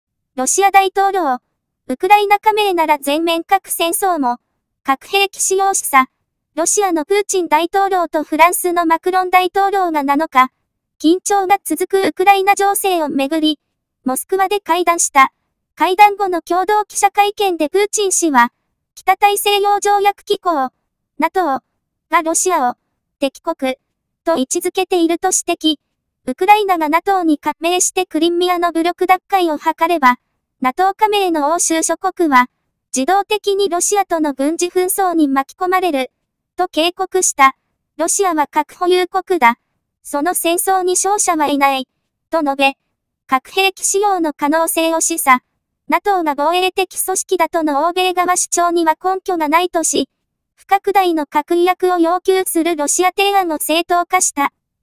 ロ大統領、全面核戦争もa
ロ大統領、ウクライナ加盟なら全面核戦争も　核兵器使用示唆